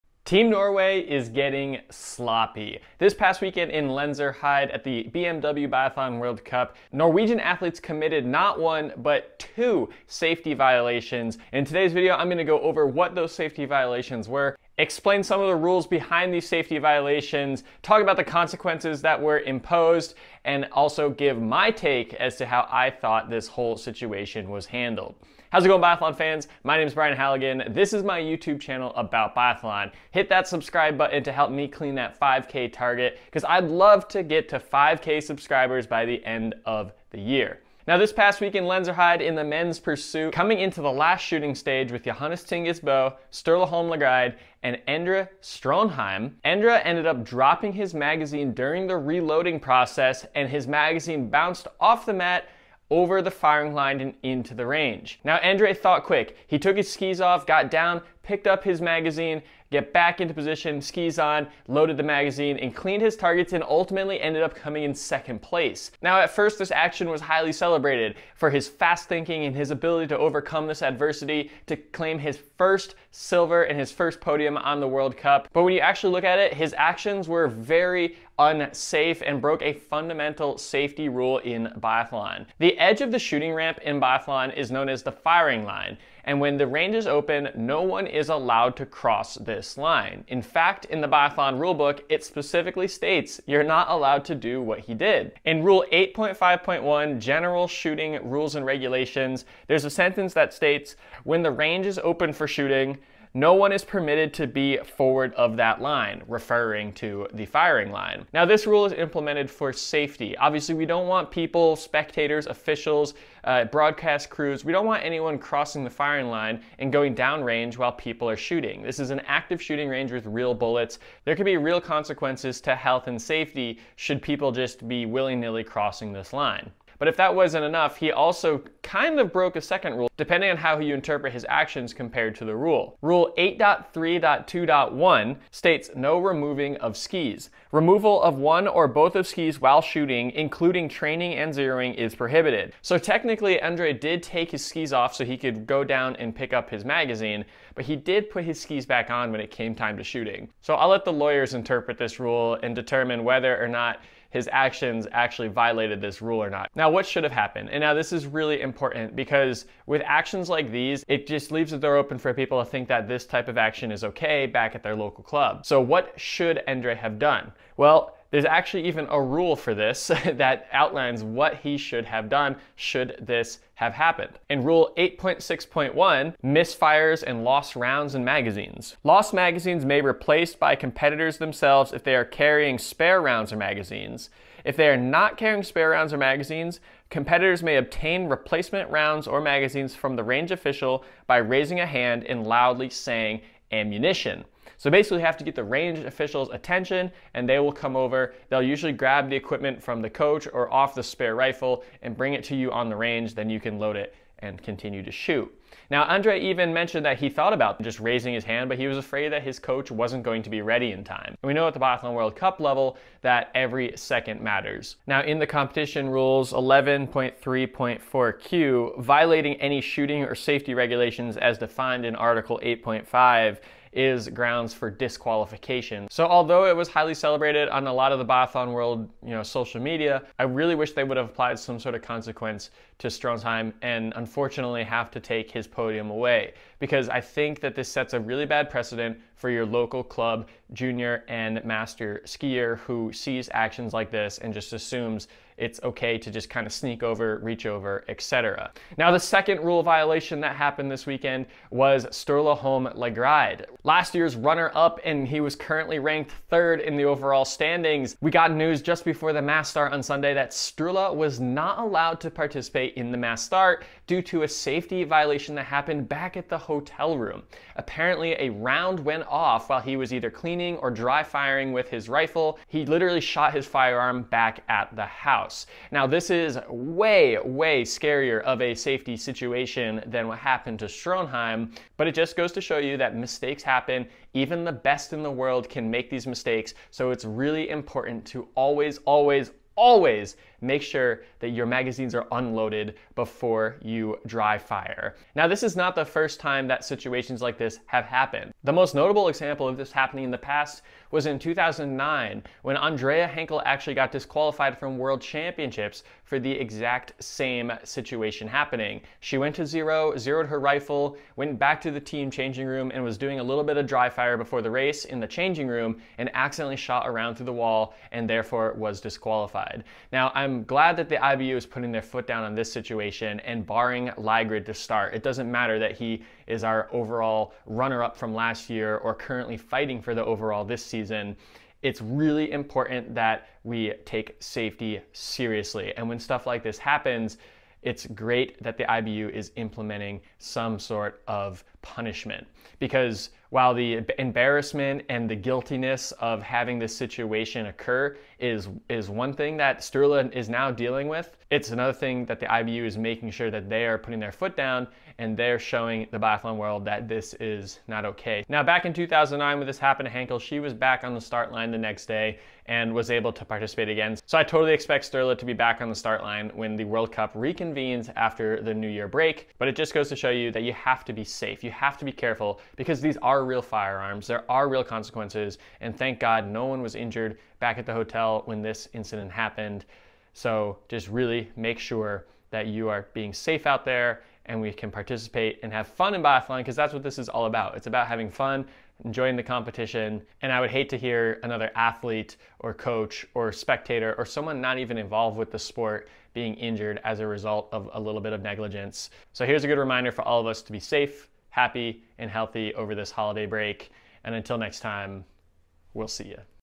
2 Biathlon Safety Violations This Weekend | Endre Stroemsheim | Sturla Holm Lægreid
As the Biathlon World Cup wrapped up the first trimester in Lenzerheide, Switzerland this weekend two Norwegian athletes committed safety violations that need to be addressed. Endre Stroemsheim and Sturla Holm Lægreid were the two athletes guilty of violations.

0:00 2 Biathlon Safety Violations
0:37 Endre Stroemsheim reaches over firing line
4:08 Sturla Holm Lægreid has rifle incident in hotel room